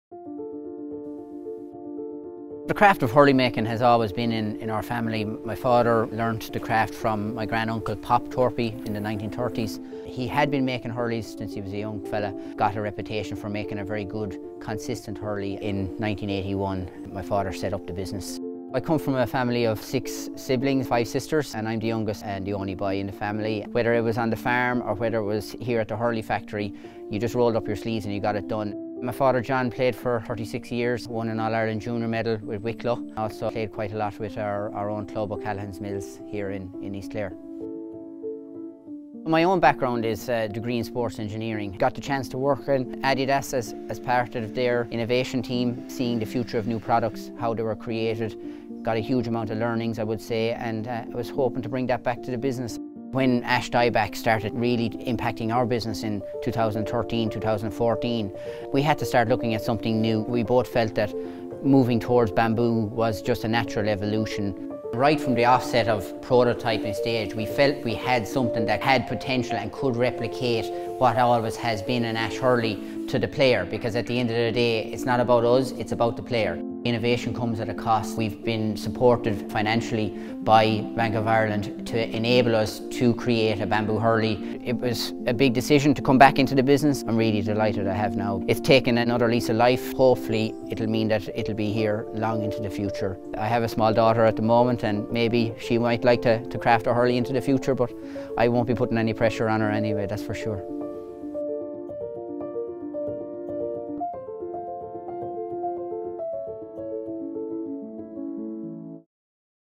The Best Businesses Never Stop Beginning - Torpey Hurleys
Torpey Hurleys was established in 1981 and is now in its’ third generation of master craftsmen.

Constantly evolving to stay in front, they developed the award-winning Bambú hurley.

Bank of Ireland, Begin.